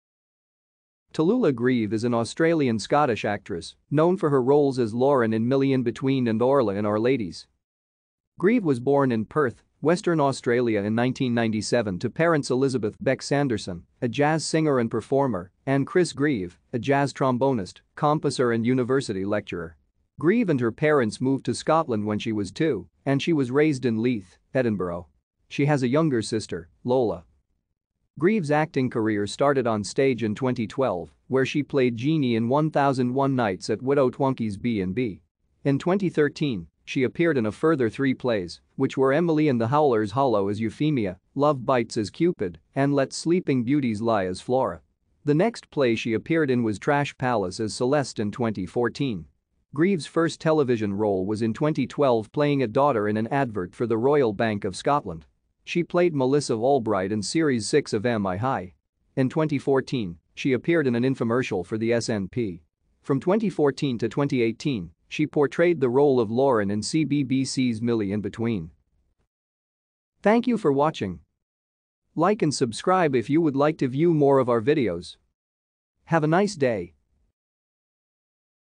Tallulah Greive
Tallulah Greive  is an Australian-Scottish actress, known for her roles as Lauren in Millie Inbetween and Orla in Our Ladies.